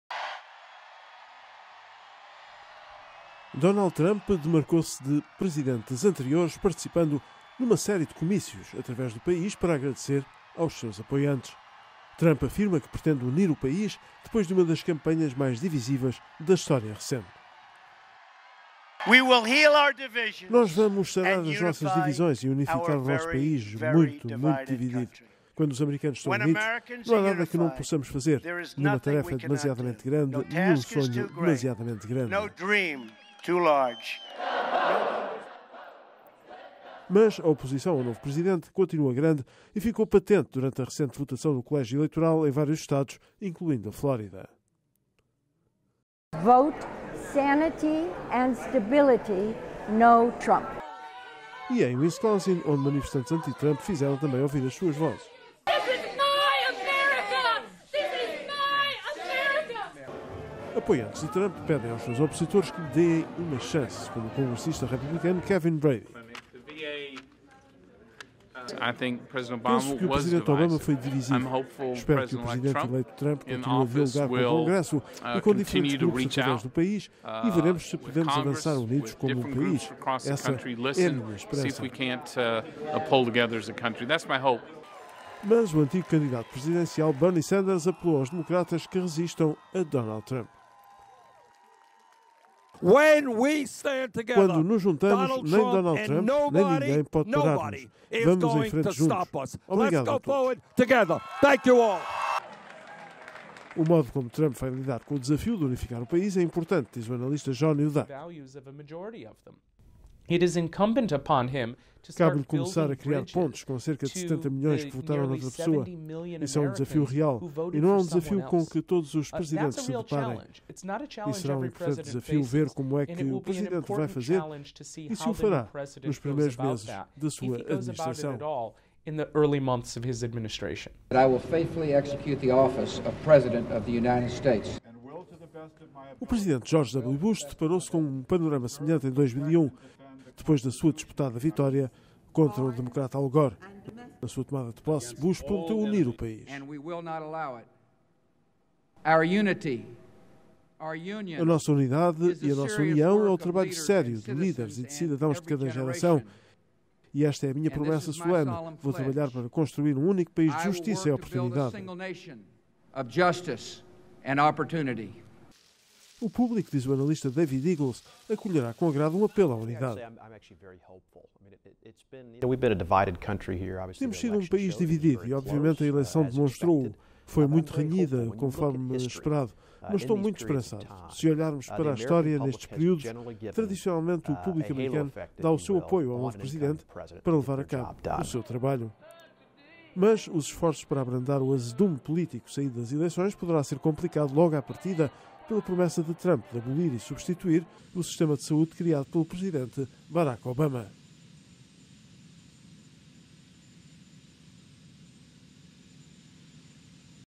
A divisão política da América
Donald Trump prometeu actuar rápido para aprovar a sua agenda executiva e legislativa no sentido de mudar Washington. Mas como presidente ele fará face a uma enorme divisão política nos Estados Unidos que permanece após a sua vitória nas eleiçōes de Novembro, sobe Hillary Clinton.